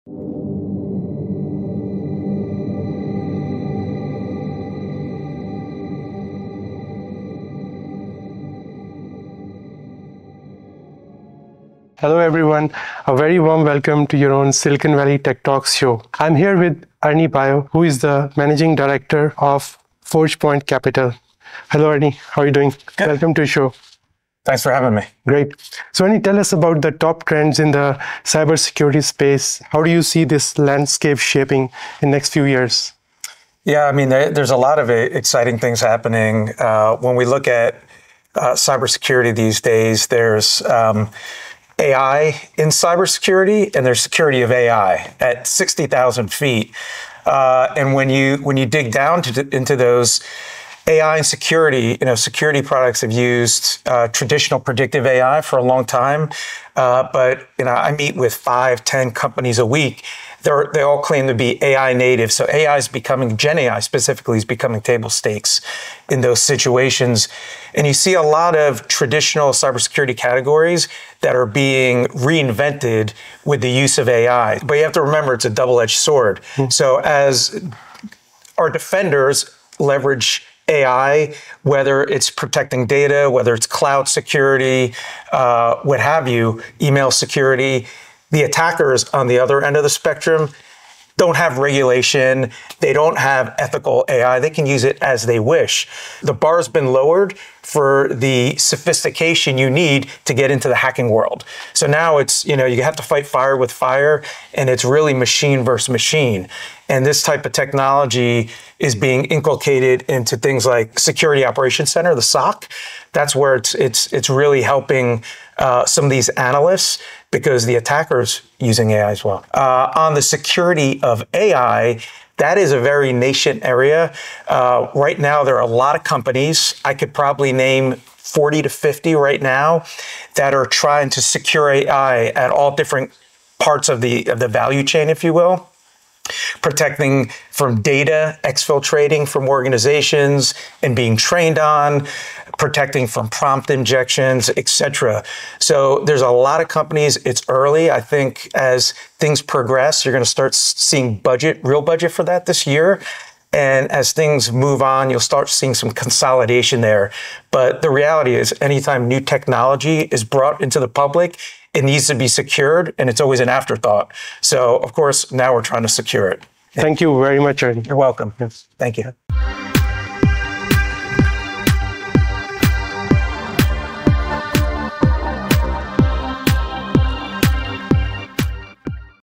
AI vs Hackers: Ernie Bio on Cybersecurity Trends & the Future of Secure AI | TechCon SoCal 2025
In this exclusive sit-down at TechCon SoCal 2025, we talk with Ernie Bio, Managing Director at Forge Point Capital, about the future of cybersecurity, the rise of AI-native startups, and why security of AI is the next big battleground.

Discover how defenders are using AI to stay ahead of increasingly advanced cyber threats and why bad actors are evolving just as fast. Learn about trends in Security Operations Centers (SOC), AI-based email protection, prompt injection attacks, and the urgent need for AI security frameworks.

0:00 – Intro & Guest Welcome
0:28 – Cybersecurity Trends Overview
0:48 – AI in Security Products
1:07 – AI-Native Startups & Reinvention
1:18 – The Double-Edged Sword of AI
1:42 – Machine vs Machine Warfare
2:01 – Security of AI as a New Frontier
2:28 – Budget Growth & Market Consolidation
2:39 – Why AI Security Is an Afterthought
2:45 – Closing Remarks

🔐 From "machine vs machine" combat to the business of securing generative AI this interview is a must-watch for tech leaders, CISOs, and startup founders.

📅 Don’t miss out! Watch now and stay ahead of the tech curve!